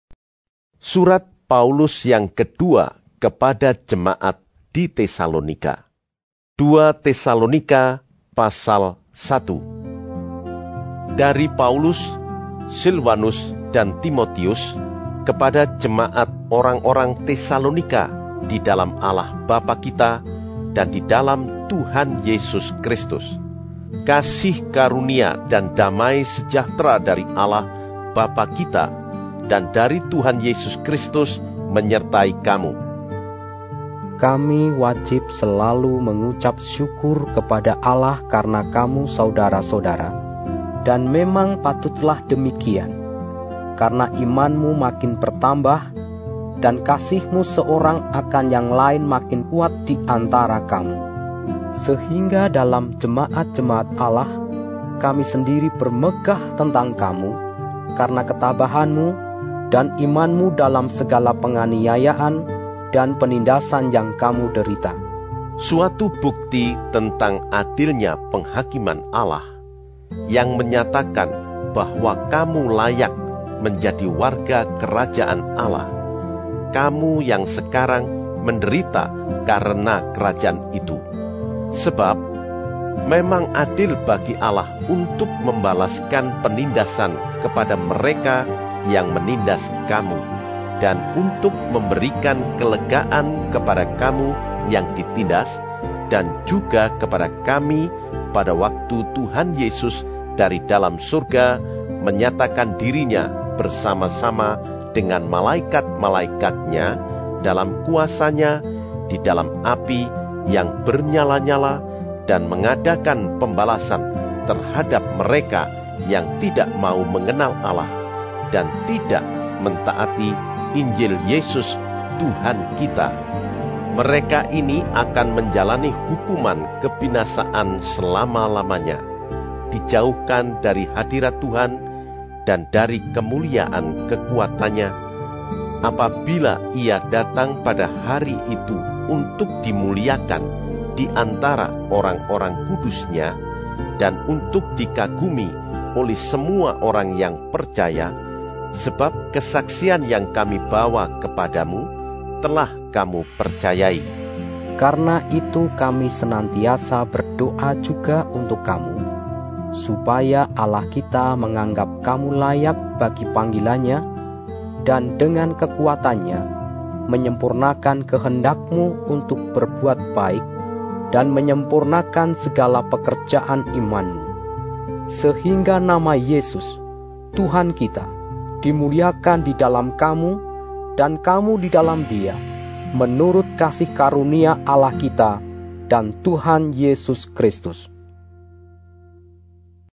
Alkitab Audio - 2 Tesalonika 1
2 Tesalonika 1
Salam
1:1 Dari Paulus, Silwanus dan Timotius, kepada jemaat orang-orang Tesalonika di dalam Allah Bapa kita dan di dalam Tuhan Yesus Kristus. 1:2 Kasih karunia dan damai sejahtera dari Allah, Bapa kita, dan dari Tuhan Yesus Kristus menyertai kamu.
Ucapan syukur dan doa
1:3 Kami wajib selalu mengucap syukur kepada Allah karena kamu, saudara-saudara. Dan memang patutlah demikian, karena imanmu makin bertambah dan kasihmu seorang akan yang lain makin kuat di antara kamu, 1:4 sehingga dalam jemaat-jemaat Allah kami sendiri bermegah tentang kamu karena ketabahanmu dan imanmu dalam segala penganiayaan dan penindasan yang kamu derita: 1:5 suatu bukti tentang adilnya penghakiman Allah, yang menyatakan bahwa kamu layak menjadi warga Kerajaan Allah, kamu yang sekarang menderita karena Kerajaan itu. 1:6 Sebab memang adil bagi Allah untuk membalaskan penindasan kepada mereka yang menindas kamu 1:7 dan untuk memberikan kelegaan kepada kamu yang ditindas, dan juga kepada kami, pada waktu Tuhan Yesus dari dalam sorga menyatakan diri-Nya bersama-sama dengan malaikat-malaikat-Nya, dalam kuasa-Nya, di dalam api yang bernyala-nyala, 1:8 dan mengadakan pembalasan terhadap mereka yang tidak mau mengenal Allah dan tidak mentaati Injil Yesus, Tuhan kita. 1:9 Mereka ini akan menjalani hukuman kebinasaan selama-lamanya, dijauhkan dari hadirat Tuhan dan dari kemuliaan kekuatan-Nya, 1:10 apabila Ia datang pada hari itu untuk dimuliakan di antara orang-orang kudus-Nya dan untuk dikagumi oleh semua orang yang percaya, sebab kesaksian yang kami bawa kepadamu telah kamu percayai. 1:11 Karena itu kami senantiasa berdoa juga untuk kamu, supaya Allah kita menganggap kamu layak bagi panggilan-Nya dan dengan kekuatan-Nya menyempurnakan kehendakmu untuk berbuat baik dan menyempurnakan segala pekerjaan imanmu, 1:12 sehingga nama Yesus, Tuhan kita, dimuliakan di dalam kamu dan kamu di dalam Dia, menurut kasih karunia Allah kita dan Tuhan Yesus Kristus.